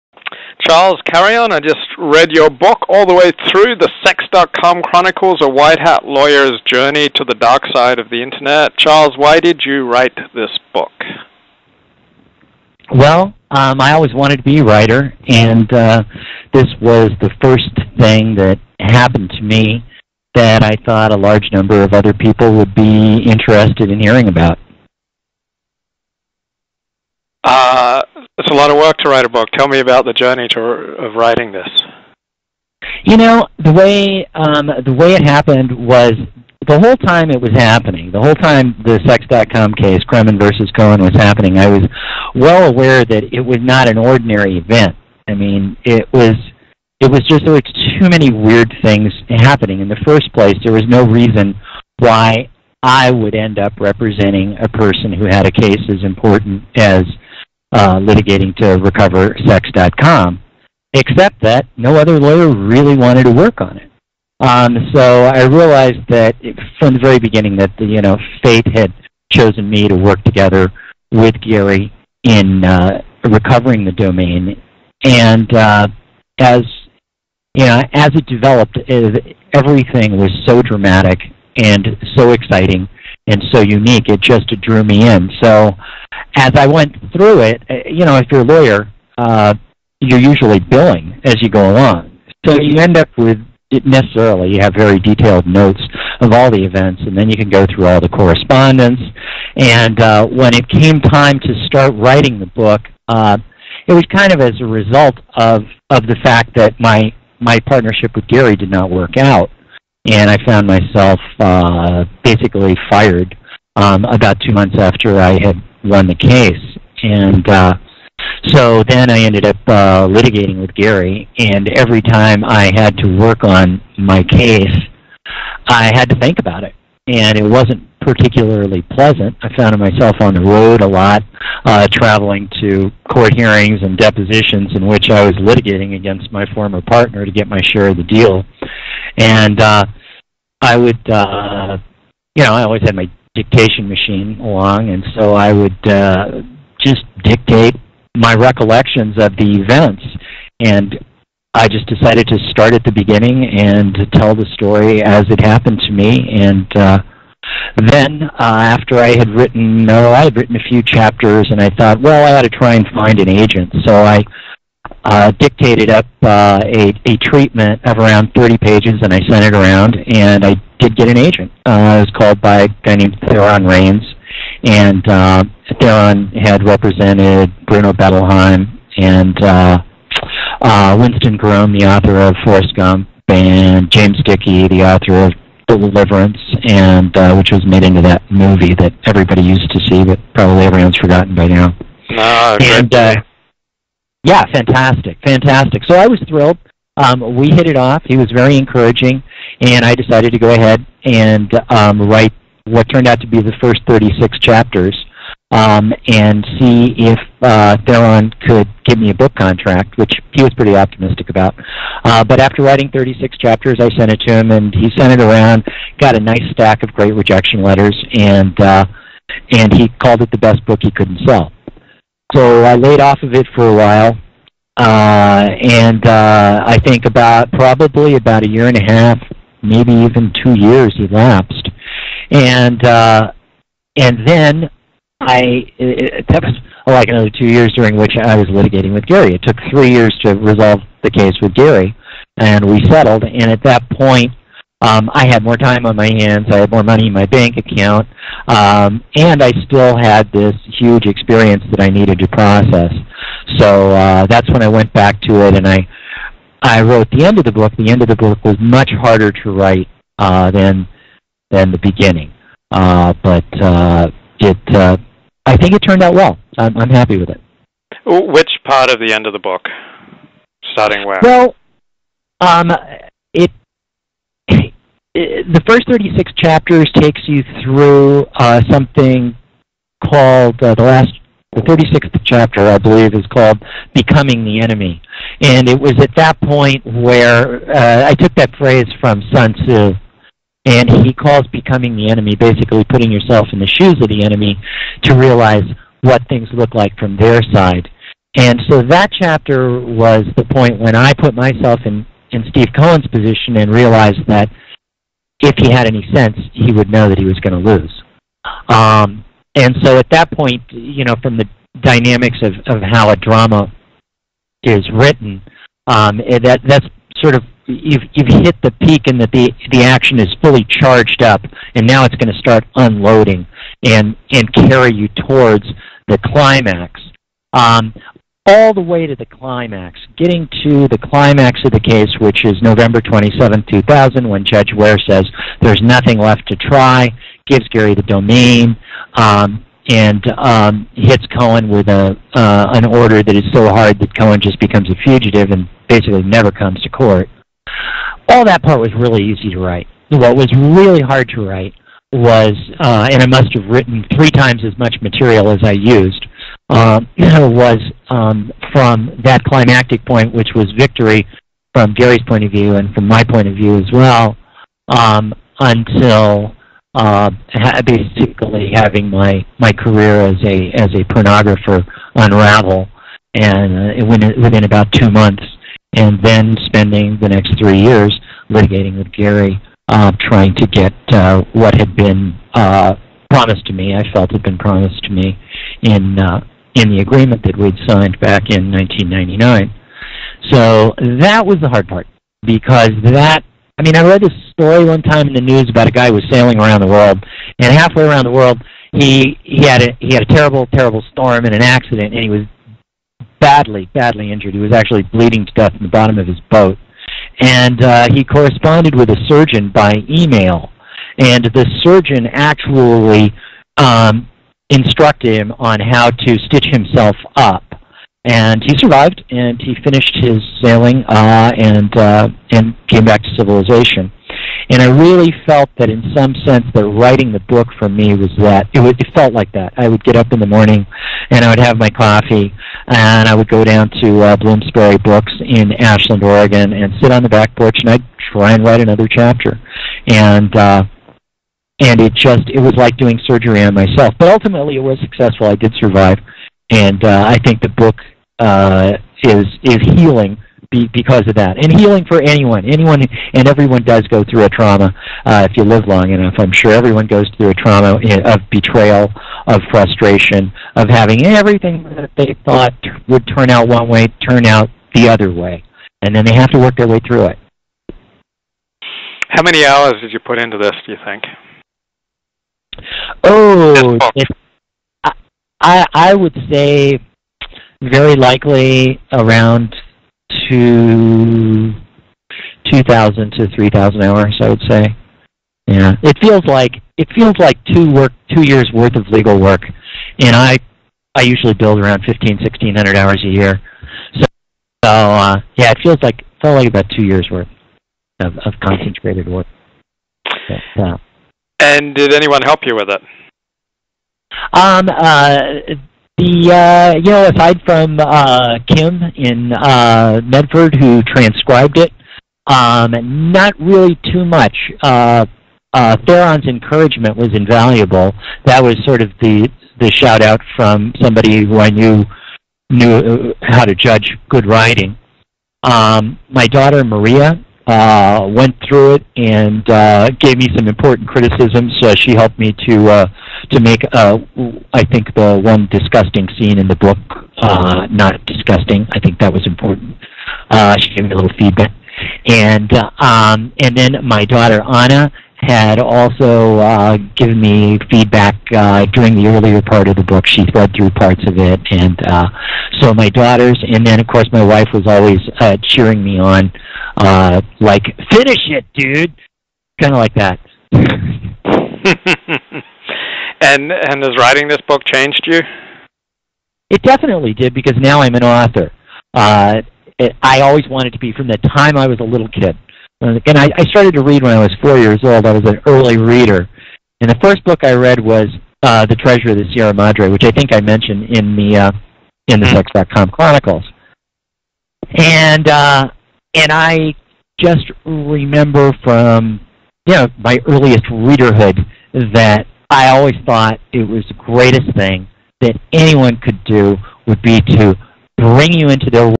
Attorney Charles Carreon Talks About His New Book: The Sex.Com Chronicles: A White-Hat Lawyer’s Jo
In 1999, I ran into Gary Kremen, and together we transformed his theoretical interest in the stolen Sex.com domain name into a reality.